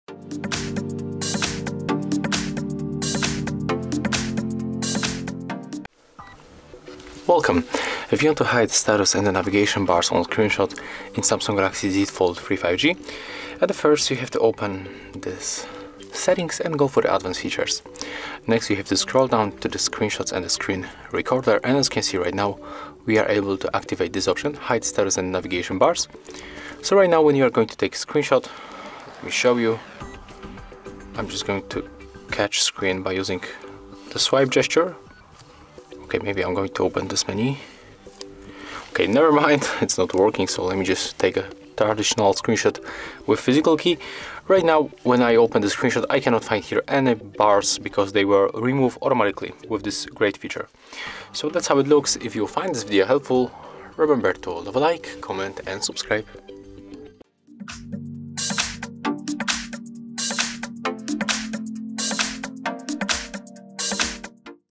How to Hide Bars on Screenshots in Samsung Galaxy Z Fold 3 5G?
In Z Fold 3 5G, you have options to hide the notification and navigation bar in the screenshot. If you edit each screenshot separately and remove these bars manually, this feature will speed up your work.

How to Remove Bars on Screenshots in Samsung Galaxy Z Fold 3 5G? How to Hide Navigation Bar on Screenshots in Samsung Galaxy Z Fold 3 5G?